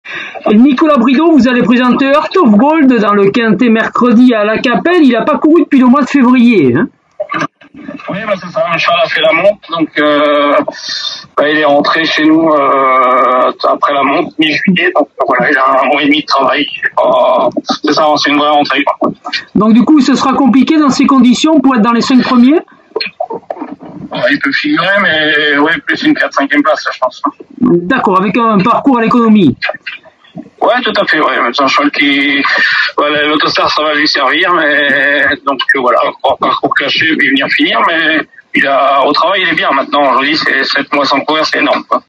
Nicolas Bridault, entraîneur de Heart of Gold (06/09 à La Capelle)
Nicolas Bridault, entraîneur de Heart of Gold (4) nous parle de ses chances dans le Quinté+, le Prix du Crédit Agricole Nord-Est sur l’hippodrome de La Capelle le 06/09/2023.